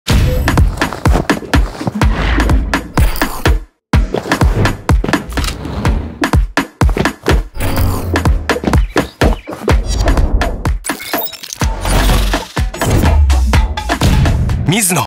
【WAVE RIDER 28】- TPE 限定款！
國民跑鞋 MIZUNO RIDER 28 全新大改款，升級的 MIZUNO ENERZY NXT 新型中底，Q彈與波浪片的穩相輔相成，讓跑動更順暢~